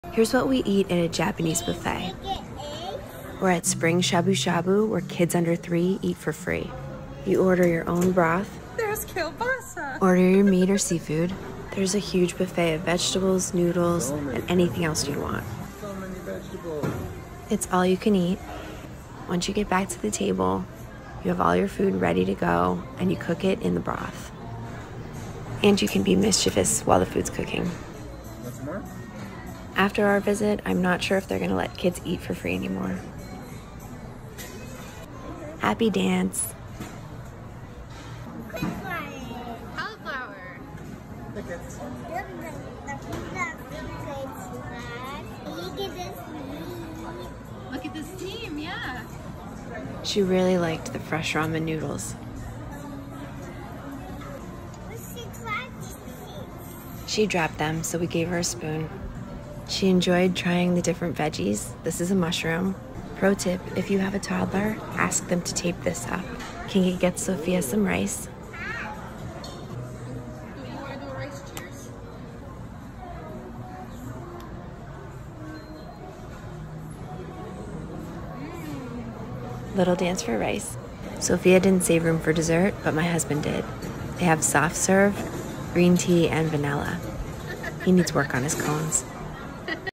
Are they sure they want to let her eat for free? 😁 Spring Shabu-Shabu Japanese Buffet - Queens, NYC
Kids under 3 eat for free! Everything we ate during our visit to Spring Shabu-Shabu, an all-you-can-eat buffet in Flushing NY. This buffet offers different flavors of broth and you cook your food in the broth right at your table. Family lunch together with my husband, toddler and our friend/babysitter, Kinga.